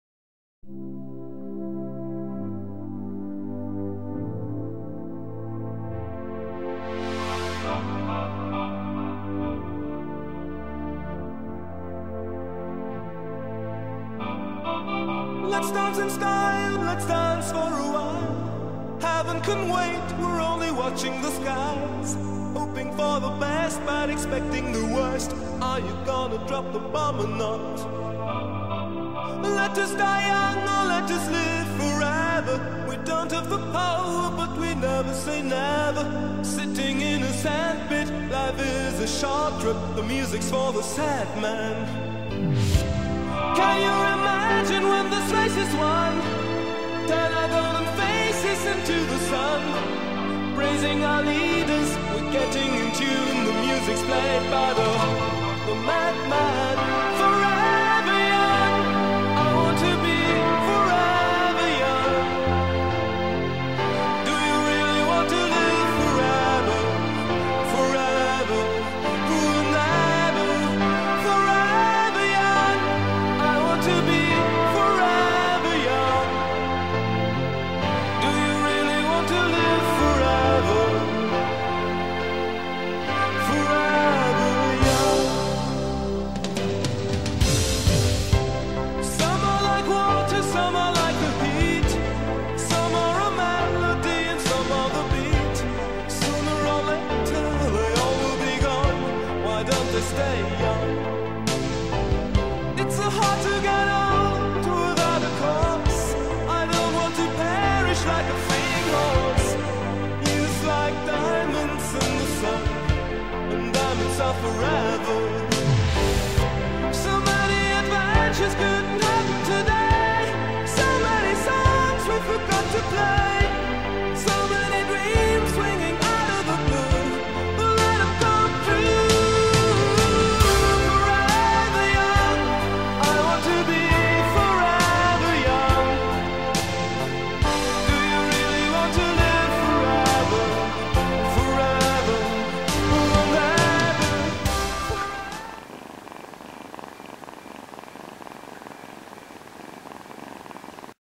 Mist, The Motion Picture_0001.wmv
Mist is the last of five kittens to be adopted from Night Pelt's late August litter - and we're hoping to find her a loving home.